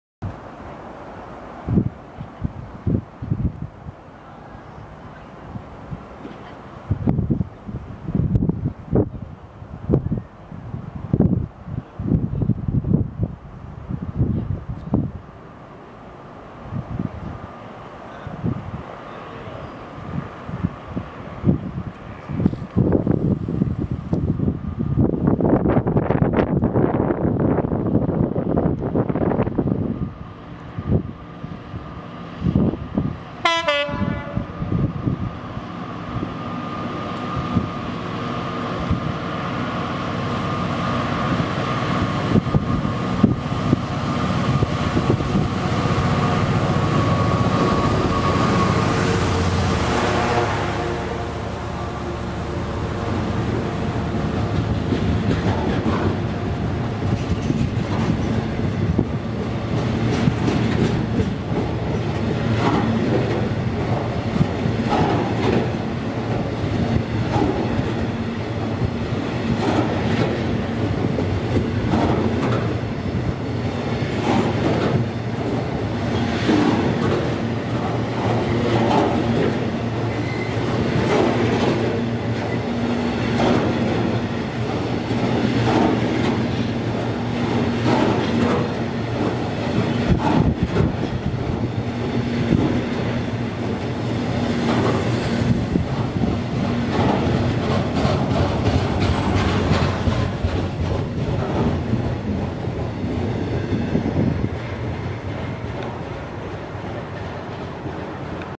DB Schenker Class 66 passes Durham (24/3/14)
With a two tone, DB Schenker Class 66 66168 hauls a freight service through Durham at low speed.

24th March 2014.